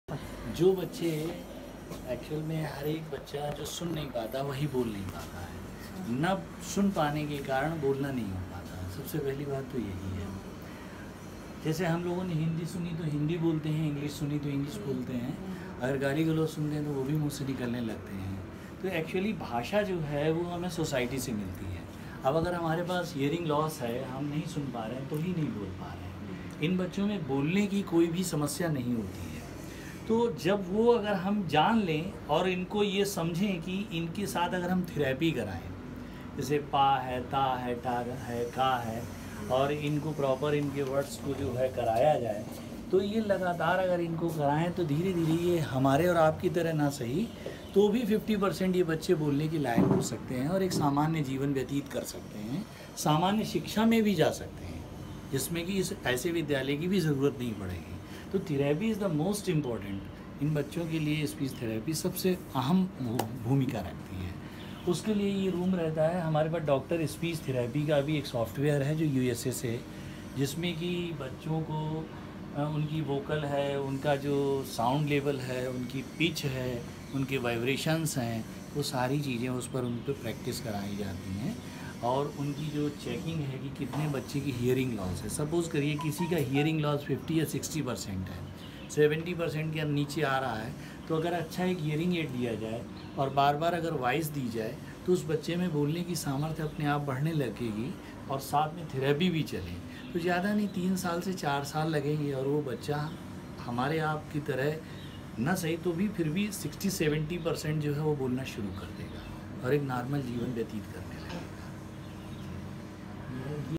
Mute's Voice
Language comes from society..
from Voice therapy even mute person can almost speak in years ..
:- a small message given by chairman of Divyang Vidyalaya (mute & deaf children's school), Saket Nagar, Kanpur (U.P) - INDIA